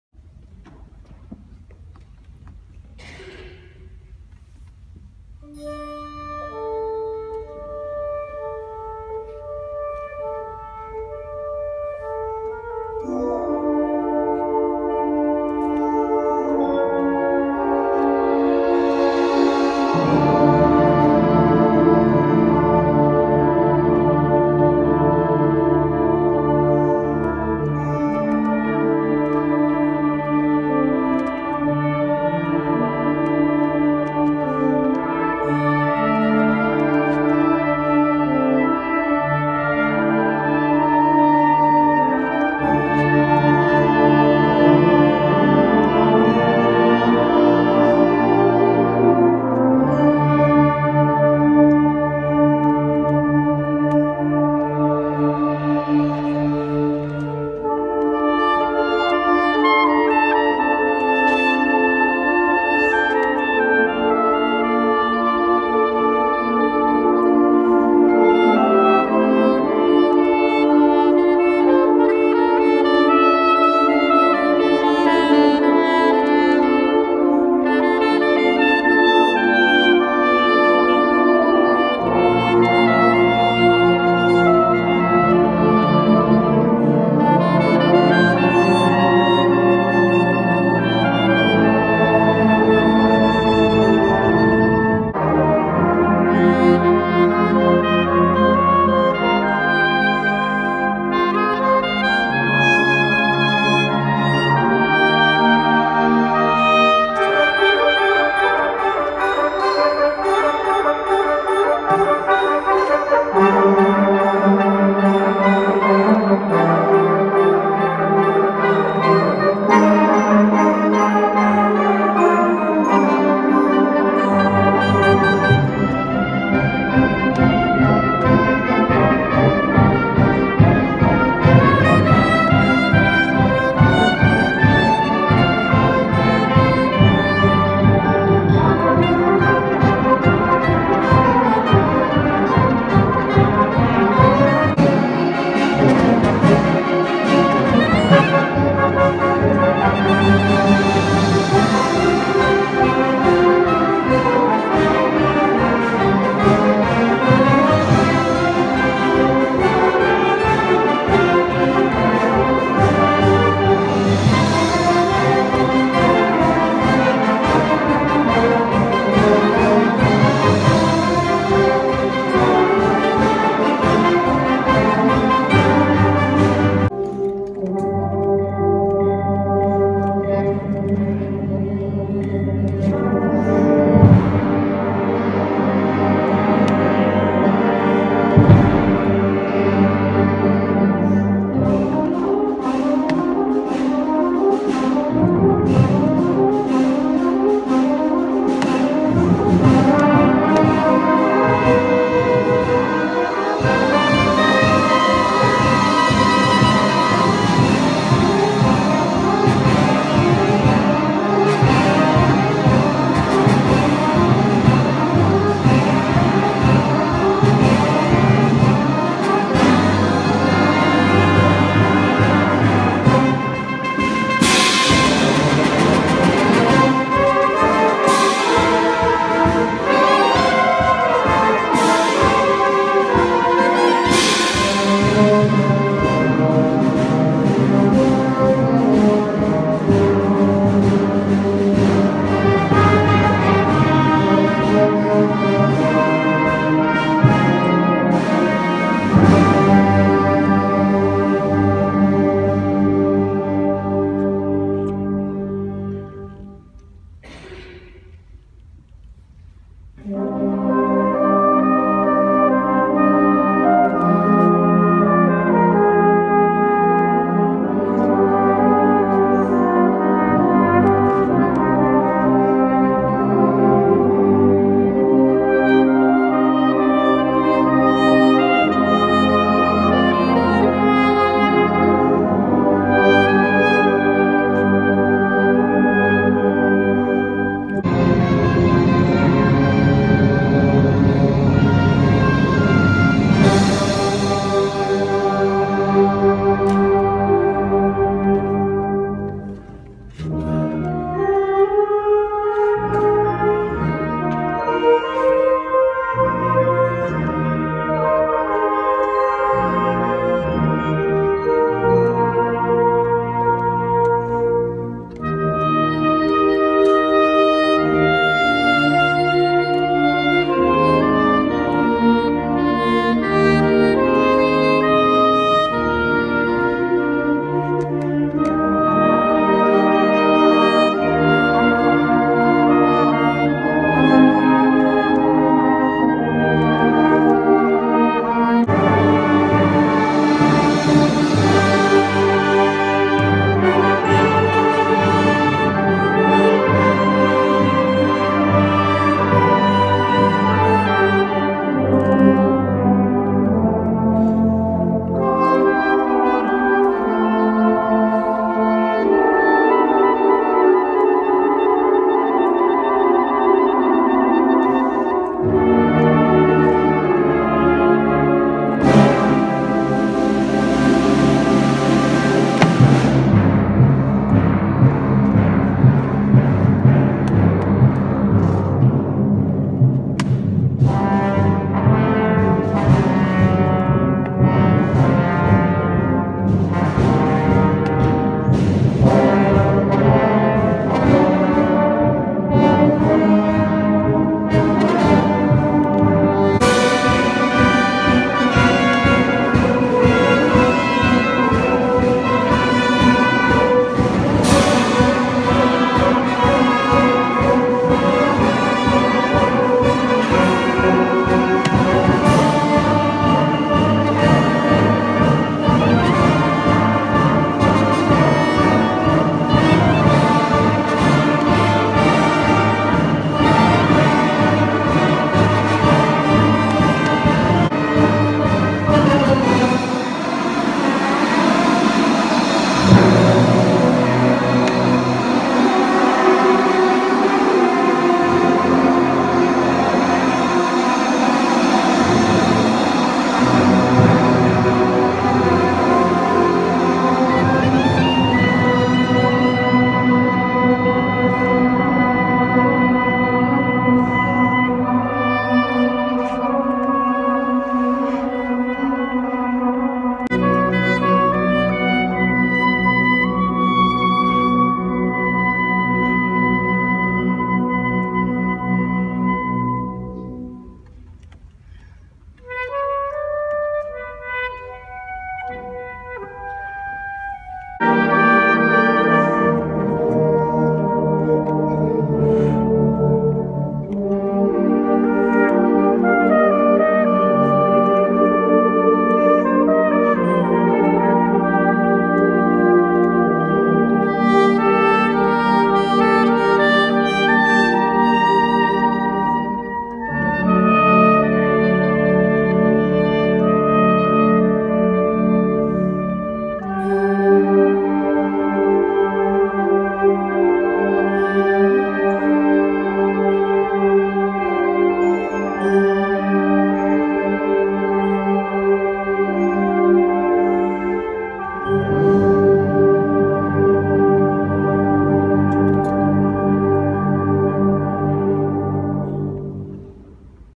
4 mei 2017 Crescendo Oostzaan
Anne Frank - herdenkingsconcert